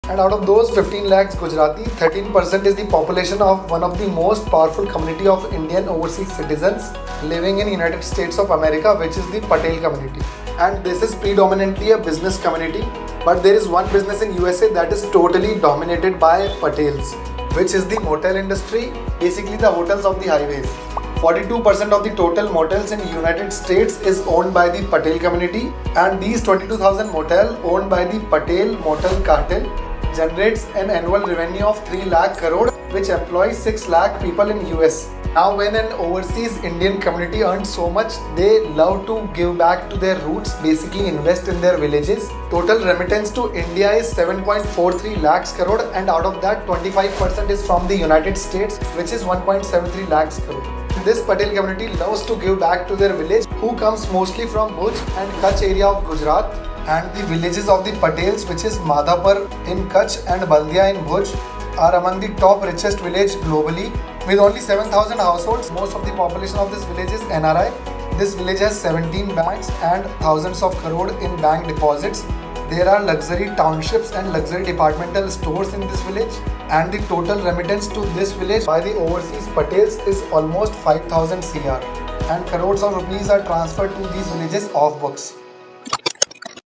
Gujarati Power in the US: 47 Lakh Indians Proving Their Might
Did you know that the Gujarati community is the most powerful among the 47 lakh overseas Indians in the United States of America? With their remarkable work ethic and vast entrepreneurial spirit, they continue to make an impact on the country's economy.

@shunyatax_global | 1000 days : 1000 vids |